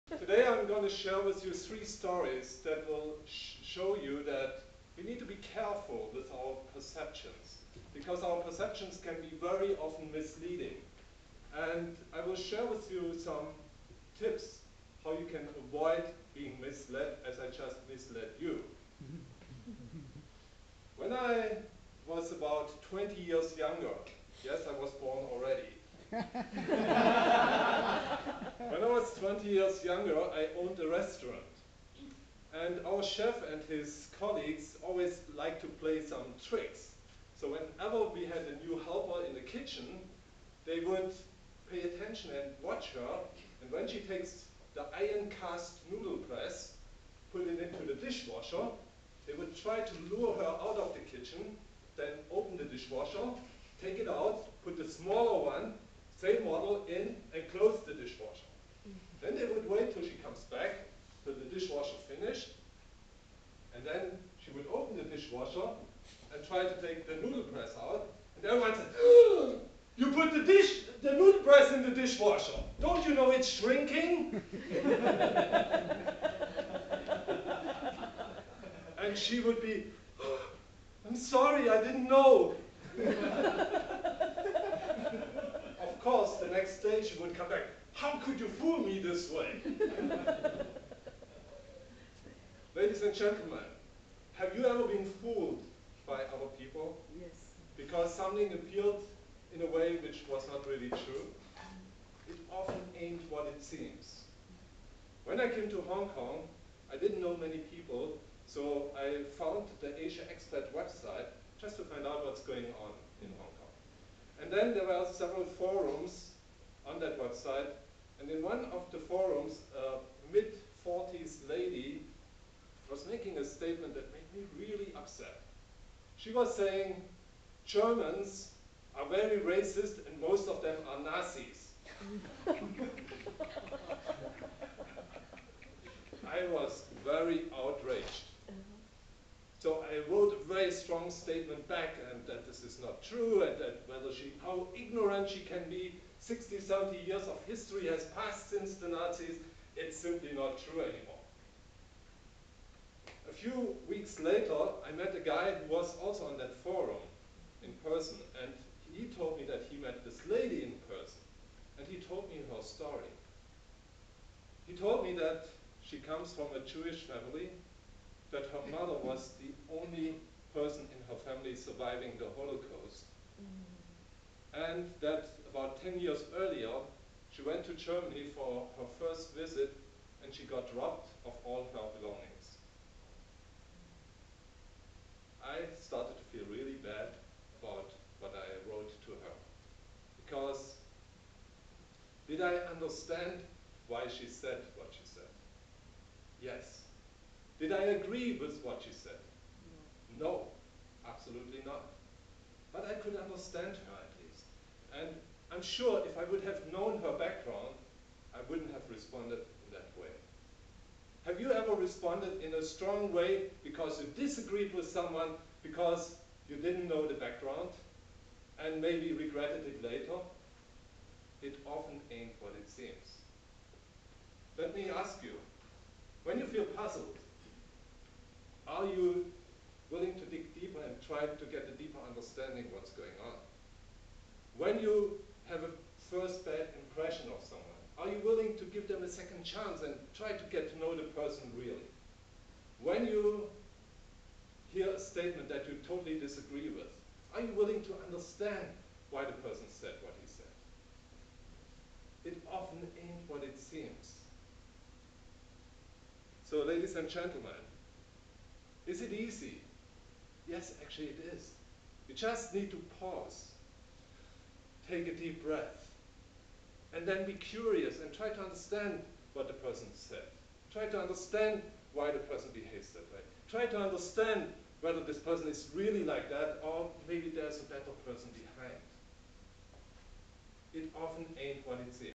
Charlie Lang's Contest Speech (2nd place winner) - Feb 2012
This video contains excerpts of Charlie's contest speech with the title: "It often ain't what it seems". He was the first runner-up to the winner Brian Hodgsen.

Charlie's speech inspires us to be more careful with our assumptions and judgments.